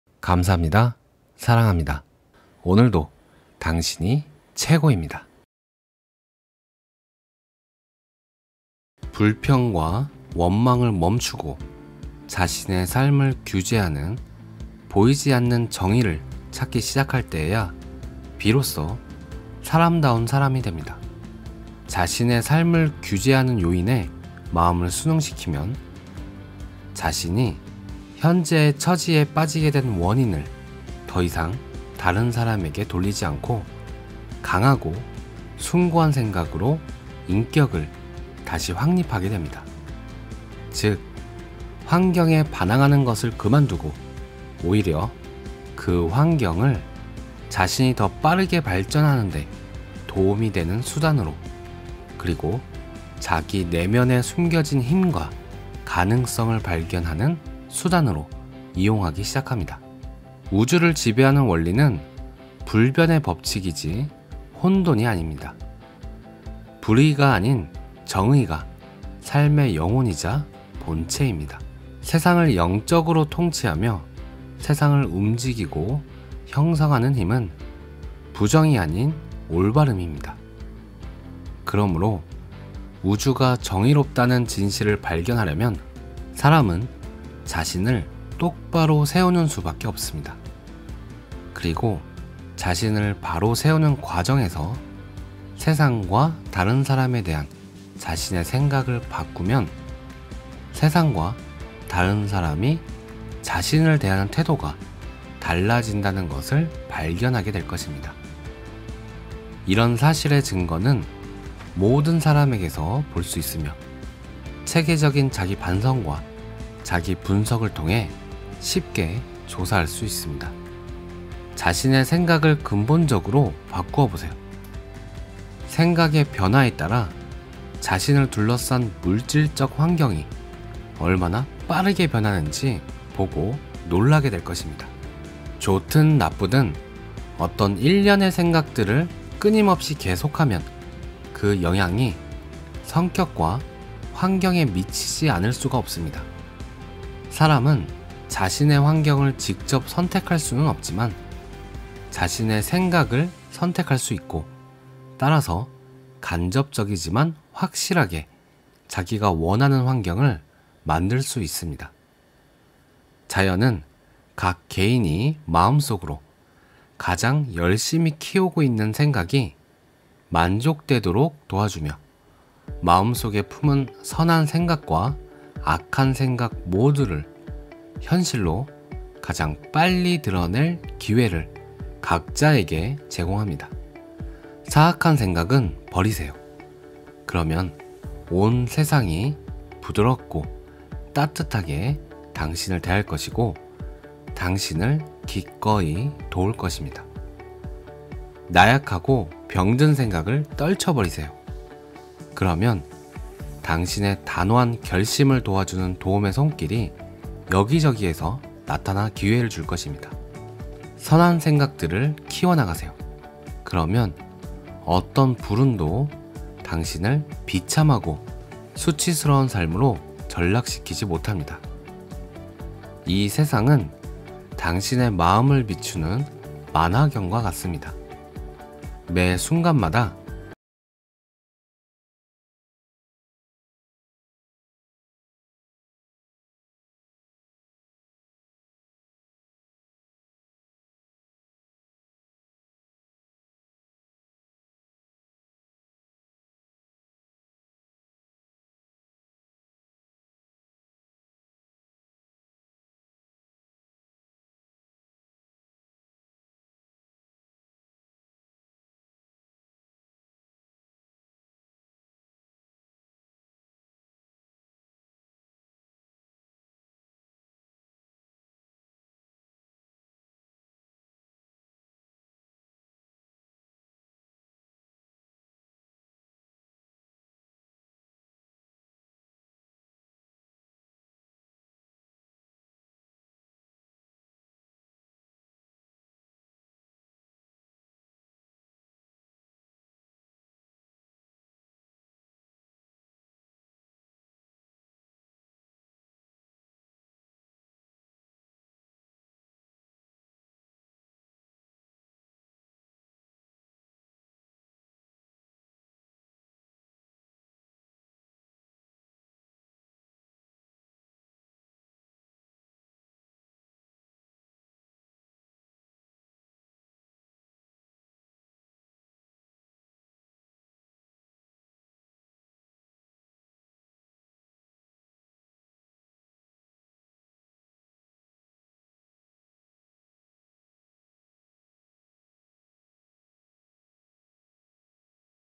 인간의 의지는 불멸의 영혼이 낳은 자식이다 03 | 생각의 지혜, 제임스 앨런
자신의 생각을 근본적으로 바꾸어 보세요. 생각의 변화에 따라 자신을 둘러싼 물질적 환경이 얼마나 빠르게 변화하는지 보고 놀라게 될 것입니다.

* 댓글과 좋아요 감사합니다.
* 써니즈 구독자님 사랑합니다.

참고서적
생각의 지혜 - 제임스 앨런